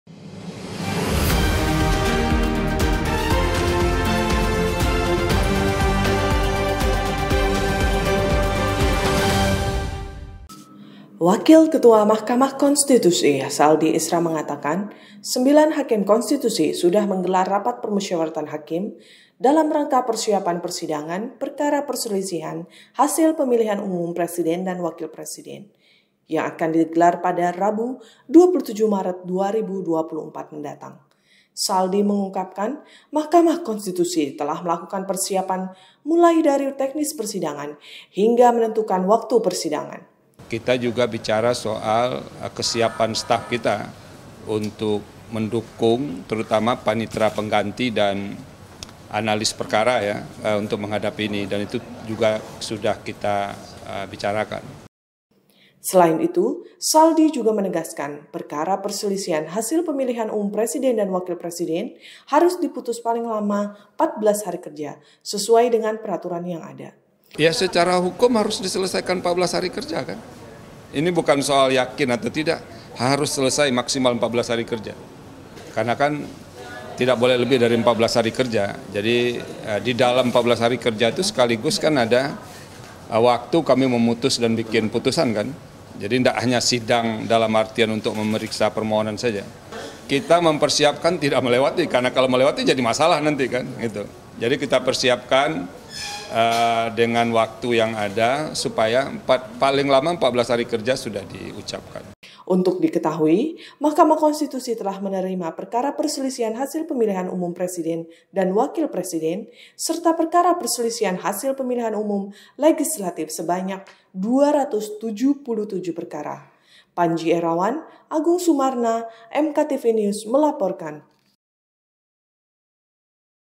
MK SIAP HADAPI PERSIDANAGN PILPRES
JAKARTA, HUMAS MKRI – Hakim Mahkamah Konstitusi (MK) menggelar Rapat Permusyawaratan Hakim (RPH) dalam rangka persiapan sidang pemeriksaan pendahuluan Perselisihan Hasil Pemilihan Umum Presiden dan Wakil Presiden (PHPU Presiden) Tahun 2024, pada Senin (25/3/2024).  Sebagaimana Peraturan MK Nomor 1 Tahun 2024, sidang pemeriksaan pendahuluan PHPU Presiden dengan agenda memeriksa kelengkapan dan kejelasan materi permohonan serta memeriksa dan mengesahkan alat bukti Pemohon akan dilaksanakan pada Rabu (27/3/2024).

“Tadi kita sudah diskusikan soal teknis persidangan dan itu kita sudah mulai menghitung hari, kapan mau untuk penyampaian keterangan dan segala macamnya karena kan (PHPU Presiden) tidak boleh lebih dari 14 hari kerja. Jadi, di dalam 14 hari kerja itu sekaligus kan ada waktu kami memutus dan bikin putusan,” ujar Wakil Ketua MK Saldi Isra kepada awak media di Gedung 1 MK, Jakarta.

Perkara PHPU Presiden diputus paling lama 14 hari kerja sejak permohonan dicatat dalam e-BRPK (Buku Registrasi Perkara Konstitusi Elektronik (e-BPRK). Saldi mengatakan, permohonan PHPU Presiden akan dicatat dalam e-BRPK pada hari ini.  Dengan demikian, MK menjadwalkan sidang pengucapan putusan PHPU Presiden pada 22 April 2024 mendatang.

“Sore ini akan diregistrasi, lalu nanti akan di-upload permohonannya,” kata Saldi.

MK akan menyampaikan salinan permohonan Pemohon kepada Termohon yakni Komisi Pemilihan Umum (KPU), dan kepada Pemberi Keterangan yaitu Badan Pengawas Pemilihan Umum (Bawaslu). Sementara, MK membuka pengajuan permohonan sebagai Pihak Terkait pada 25 sampai 26 Maret 2024. Berdasarkan Pasal 3 ayat (3) Peraturan MK Nomor 4 Tahun 2023, Pihak Terkait adalah pasangan calon presiden dan wakil presiden yang berkepentingan terhadap permohonan yang diajukan oleh Pemohon.

Selain itu, Saldi mengatakan, RPH juga membahas kesiapan jajaran petugas pendukung PHPU Tahun 2024 khususnya Panitera Pengganti (PP) dan Analis Perkara. Kemudian, dia menyebutkan, Hakim Konstitusi Arsul Sani ikut serta menangani PHPU Presiden tahun ini. Namun, MK akan membahas lebih lanjut keikutsertaan Arsul tersebut apabila terdapat pengajuan keberatan dari para pihak terhadap keberadaan Arsul menjadi hakim konstitusi yang memeriksa PHPU Presiden.

“Kita lihat apakah di antara pihak nanti ada yang mengajukan keberatan terhadap keberadaan Pak Arsul. Kalau ada nanti kita bahas,” ucap Saldi.

Sebagai informasi, MK telah menerima dua permohonan PHPU Presiden. Kedua permohonan ini diajukan pasangan calon nomor urut 01 Anies Rasyid Baswedan-Muhaimin Iskandar serta pasangan calon nomor urut 03 Ganjar Pranowo-Mahfud MD.

Tim Pemenangan Nasional (Timnas) Pasangan Anies-Muhaimin mendaftarkan permohonan PHPU Presiden ke MK pada Kamis (21/3/2024) pagi. Sedangkan Tim Pemenangan Nasional (TPN) Pasangan Ganjar-Mahfud mendaftarkan permohonan PHPU Presiden ke MK pada Sabtu (23/3/2024) sore.

Berdasarkan Peraturan Mahkamah Konstitusi Nomor 1 Tahun 2024 tentang Tahapan, Kegiatan, dan Jadwal Penanganan Perkara Perselisihan Hasil Pemilihan Umum Anggota Dewan Perwakilan Rakyat, Dewan Perwakilan Daerah, Dewan Perwakilan Rakyat Daerah, serta Pemilihan Umum Presiden dan Wakil Presiden (PMK 1/2024), MK terlebih dahulu akan menyidangkan PHPU Presiden dan Wakil Presiden dengan tenggang waktu penyelesaian paling lama 14 hari kerja sejak permohonan dicatat dalam Buku Registrasi Perkara Konstitusi Elektronik (e-BPRK).